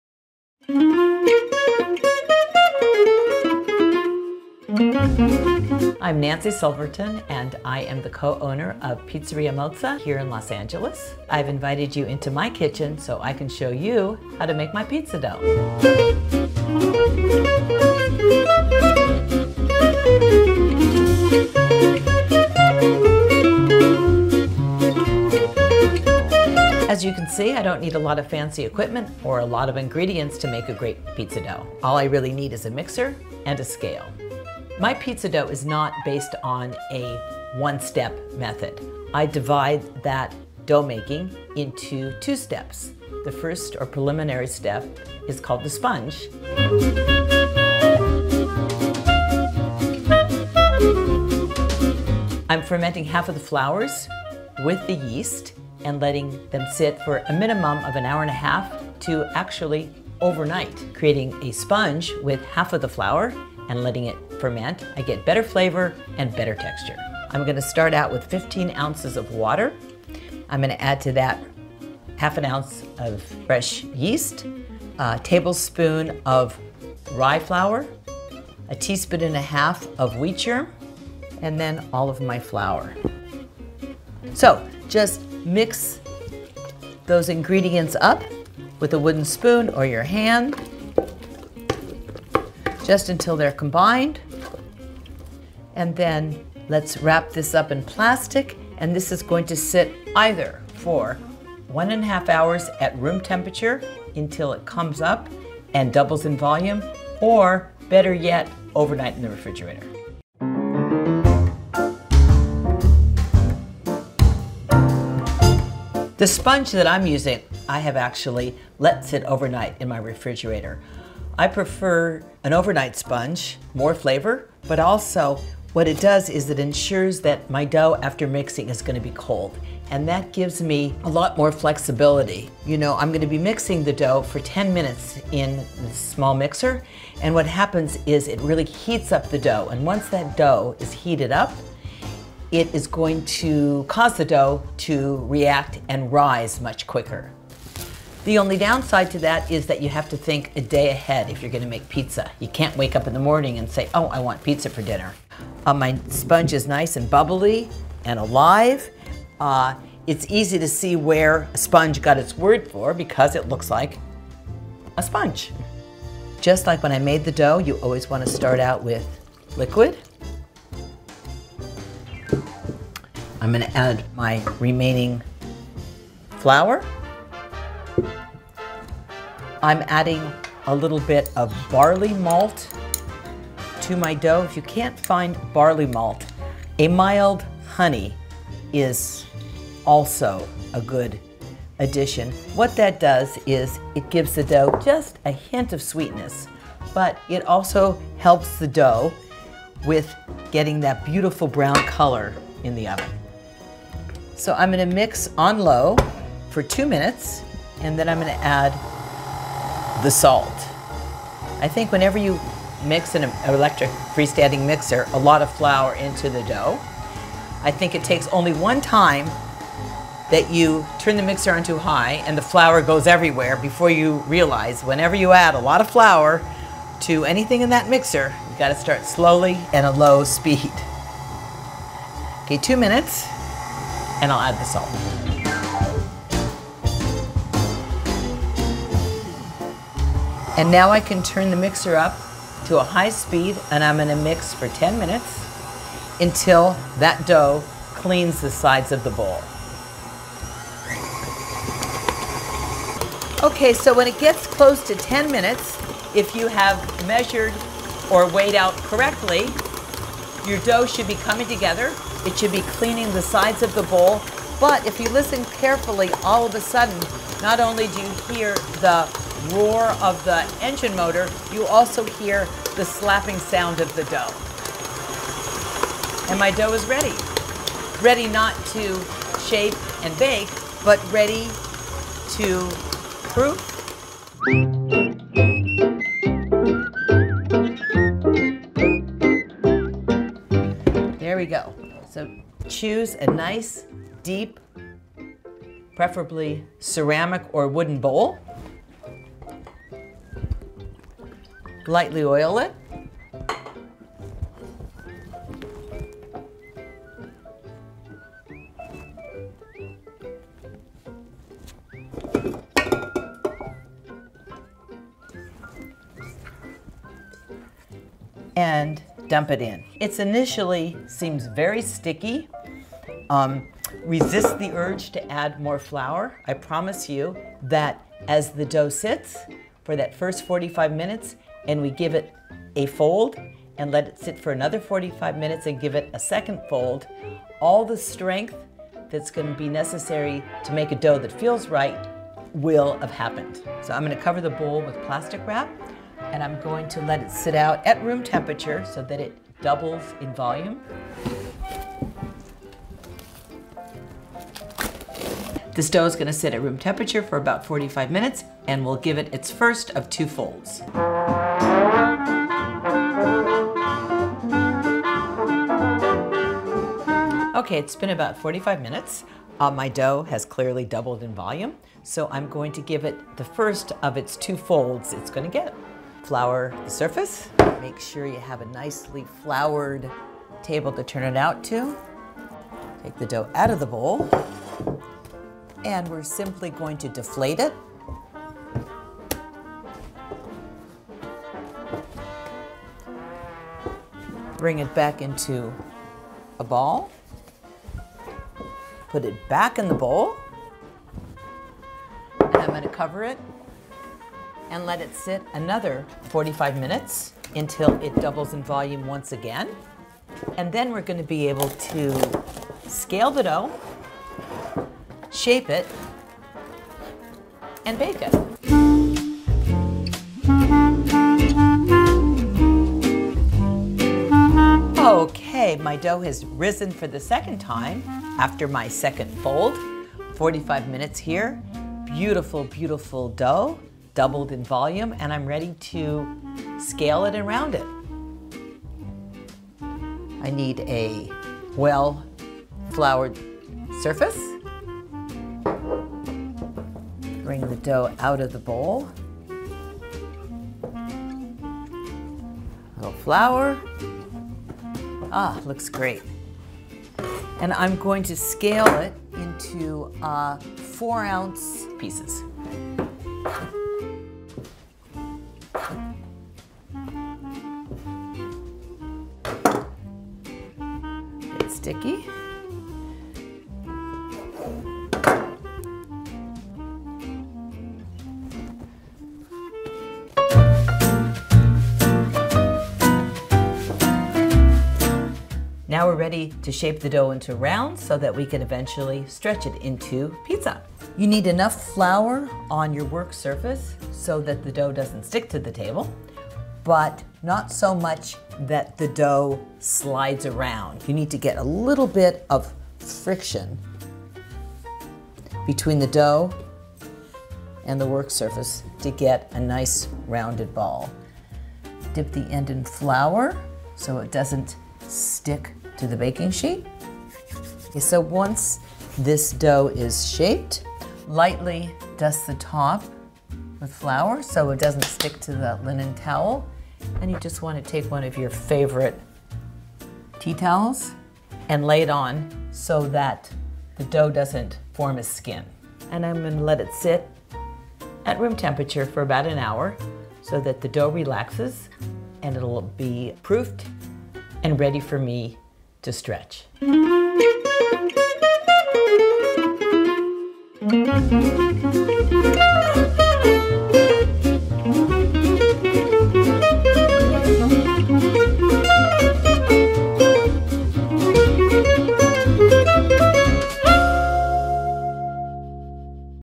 Nancy Silverton's Pizza Dough | Recipe | Ooni Pizza Ovens
Ooni Cooks| Nancy Silverton | Ooni Pizza Ovens

Welcome to the kitchen of American breadmaking trailblazer Nancy Silverton. In this episode of Ooni Cooks Nancy walks us through the full process of her world renowned pizza dough recipe.

Video chapters:
00:00 Intro
00:25 Tools & Method
00:50 The sponge
01:50 Overnight sponge
04:46 Proof
06:03 First fold
06:58 Scale and round
09:08 Ingredients list